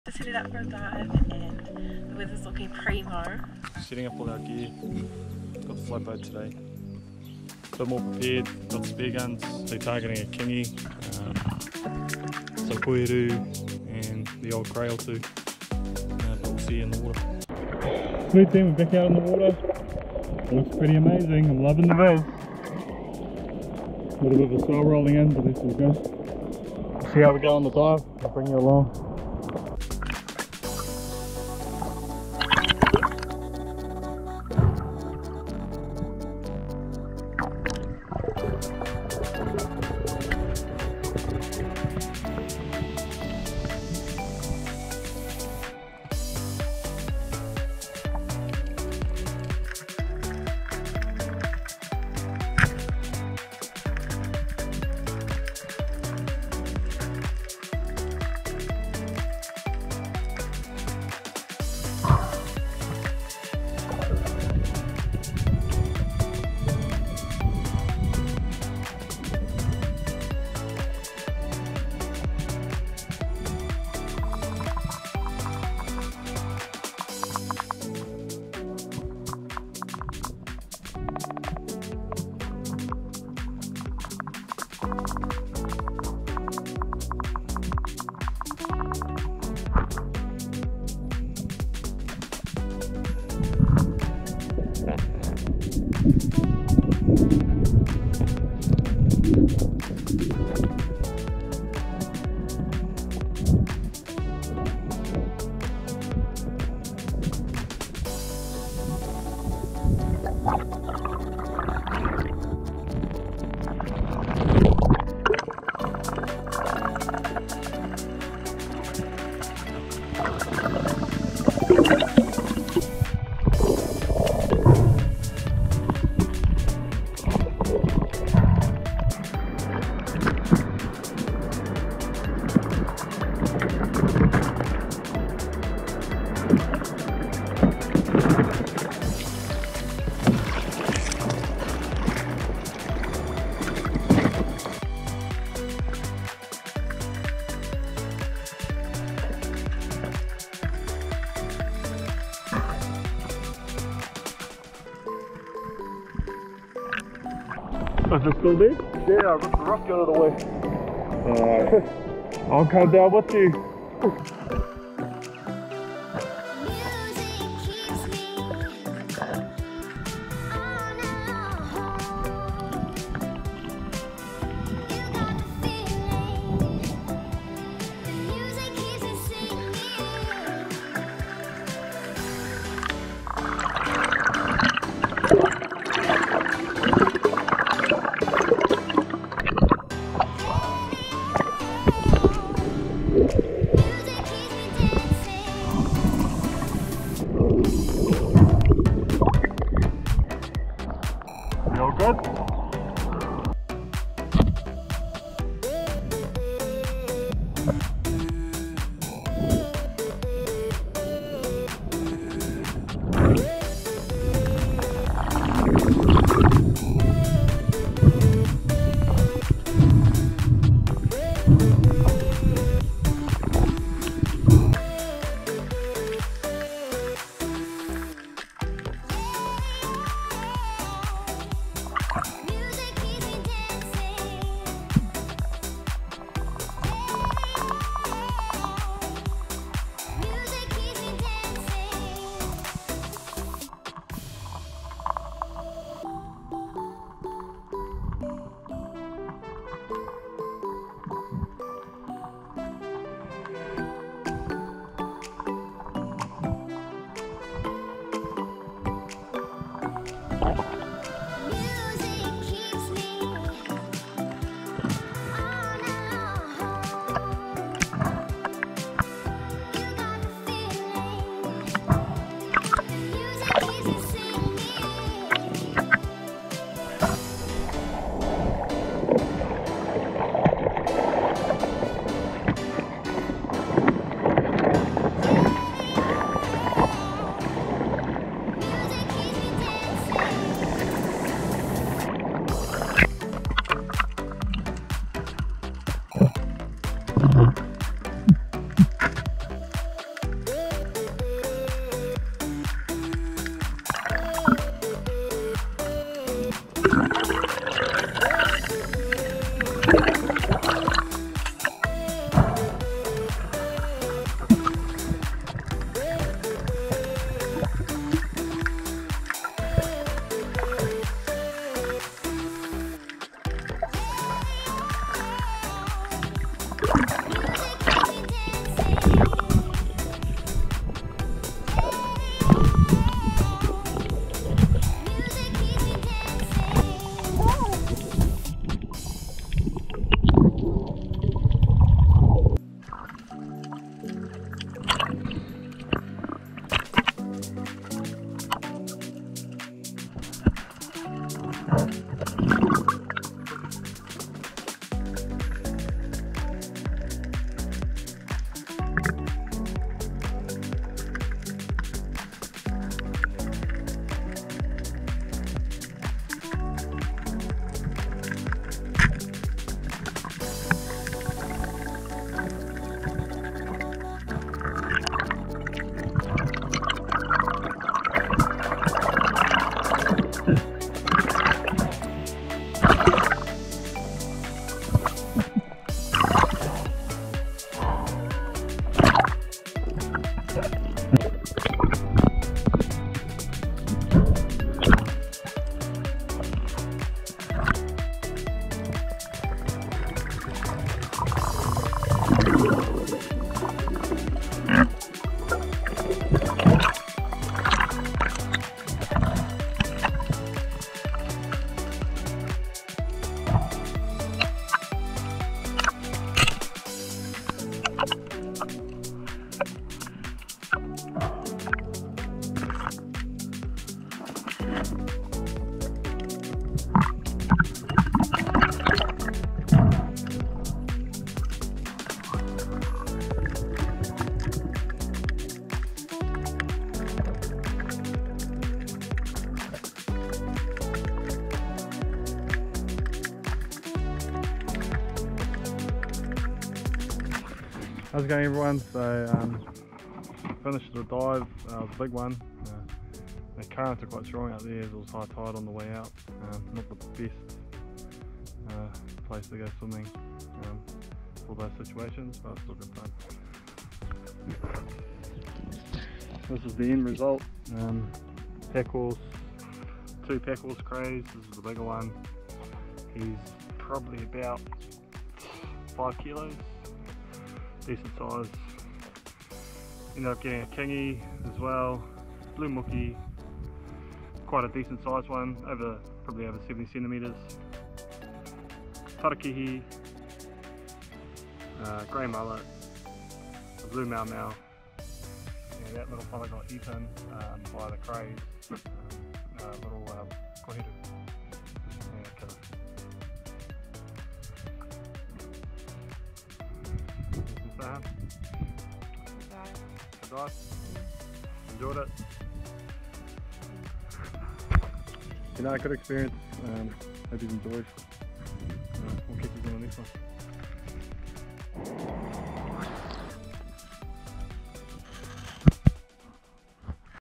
Spearfishing & Free Diving NZ BIG Crays, Kingi, Tarakihi...
Had an awesome dive in the North Island of New Zealand. Maddy, Romana and the Brother's head out for a Spearfish / Free Dive targeting Kaimoana for the Christmas kai. Big Packhorse Cray fish, Kingi, Blue Moki, Tarakihi, Grey Mullet and more.